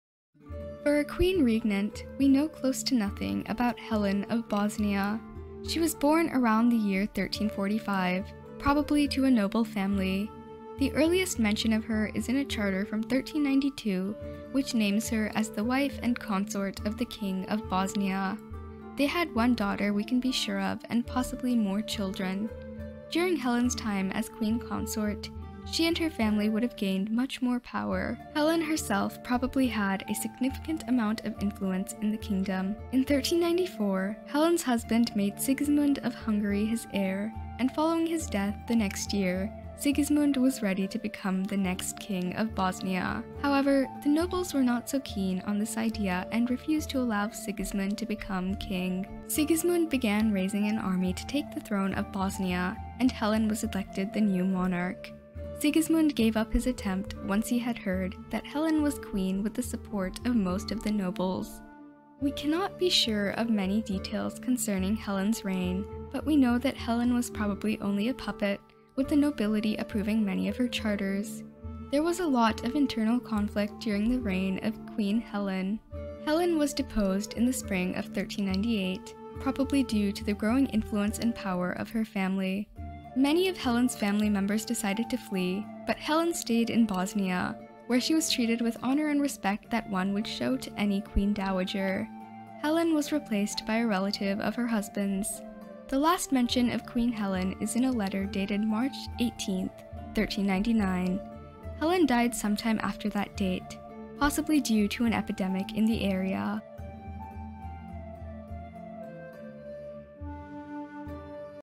Helen, Queen-Regnant of Bosnia
Helen of Bosnia was the queen-regnant of Bosnia for a few years, before being deposed. Most people have never heard of her, and we don't really know much about this 14th century queen of a not-well known kingdom.